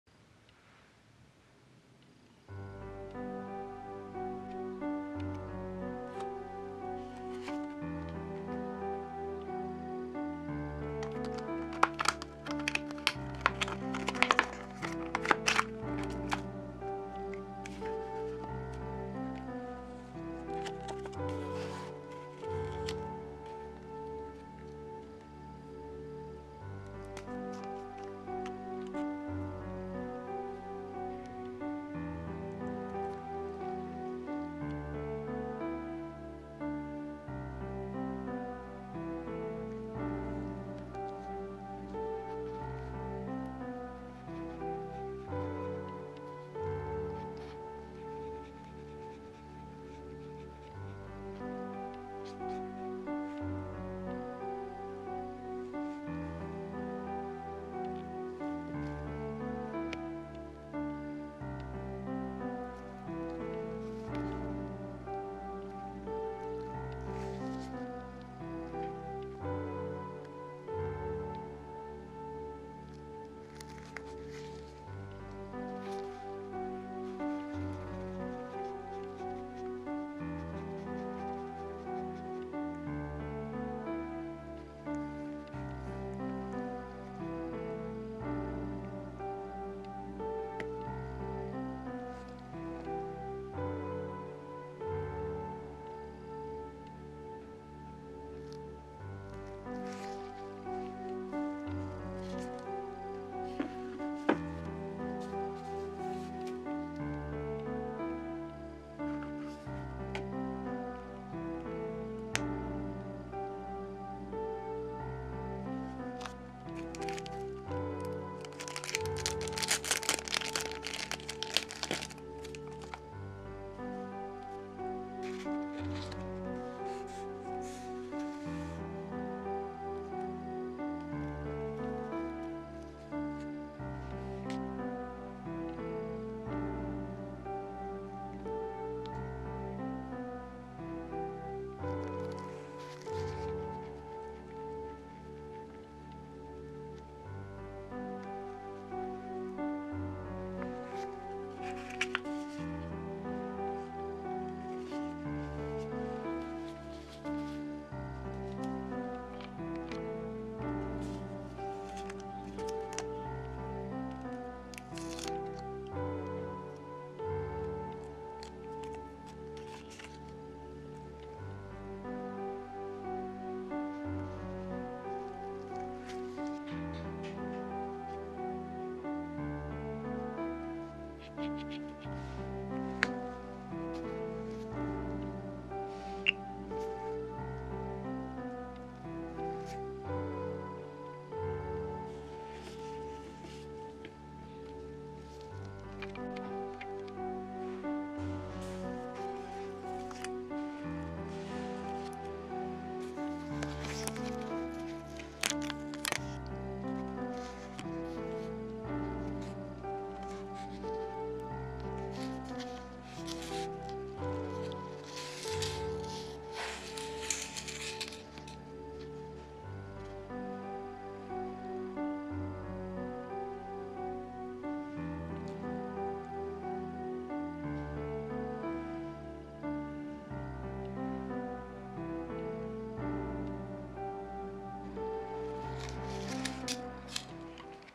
【UVレジン】時計フレームのダイヤモンドカットチャーム作ってみた
ダイヤモンドカットのモールドに着色したレジンを入れてUVランプで固めました
UV-LEDレジン、モールド、着色剤、フレームなどを使いました